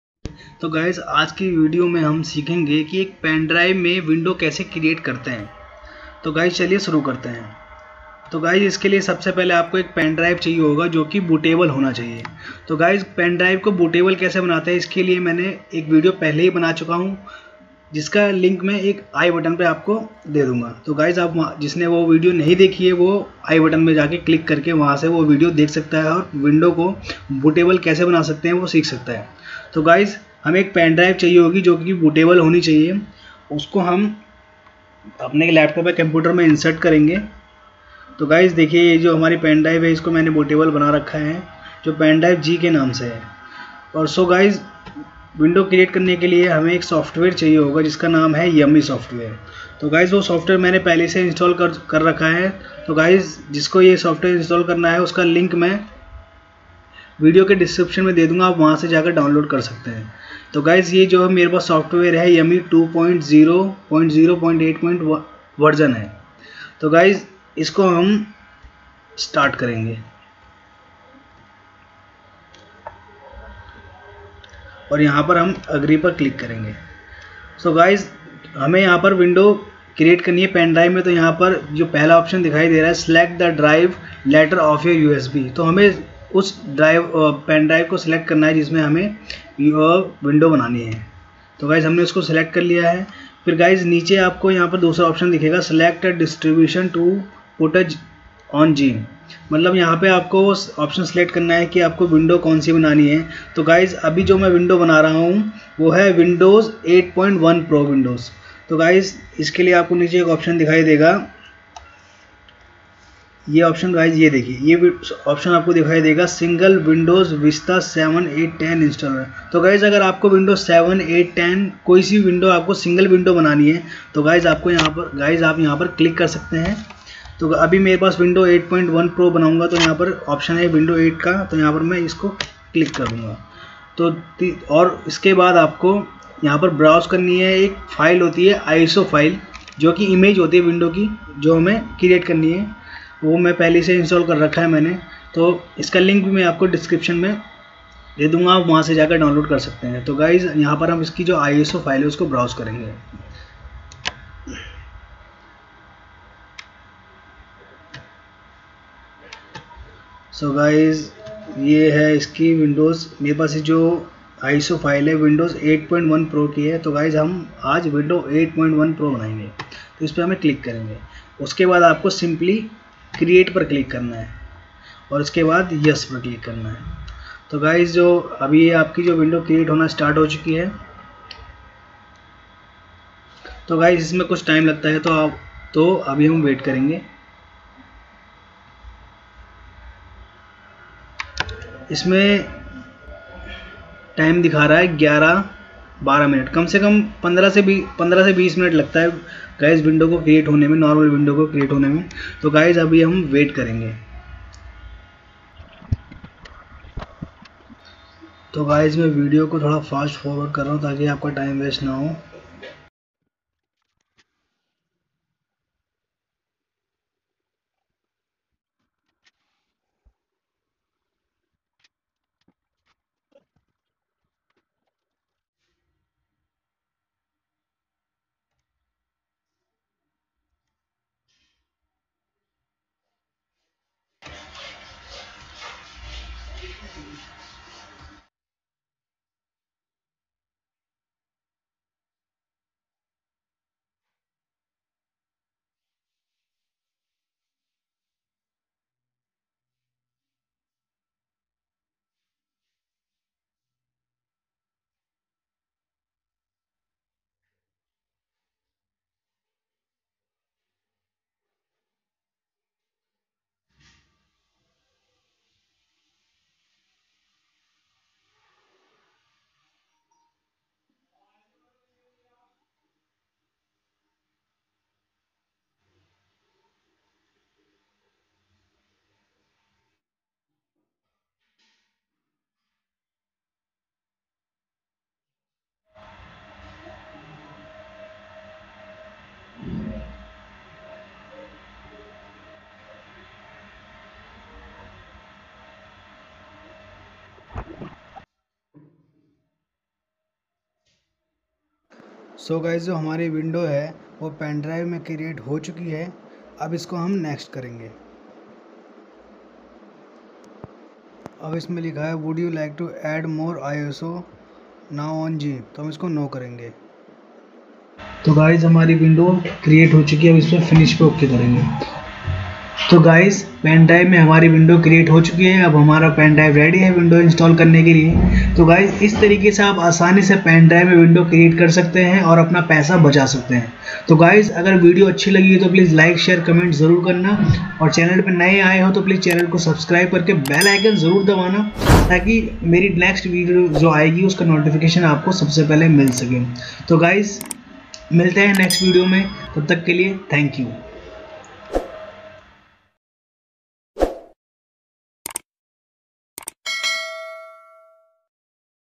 How To Create Windows8.1 pro In USB Pen drive In 2021. Make USB Driver of Windows 8.1 pro, in 2021
Hello  guys kaise ho aap sab. To   guys is video  me maine bataya hai ki agar apke pass pendrive hai or app bahar se windows buy nahi karna chahte to app khud ki ghar bethe windows create kar sakte hai. Uske liye apko Ek software jiska name YUMI  hai or ek  ISO  image of window, Jo  windows app create karna chahte hai. Description me apko uska link mil jaega niche. Waha s download kar sakte hai. baaki video ko follow  karte jao aap khud hai windows  create kar  lenge pendrive me.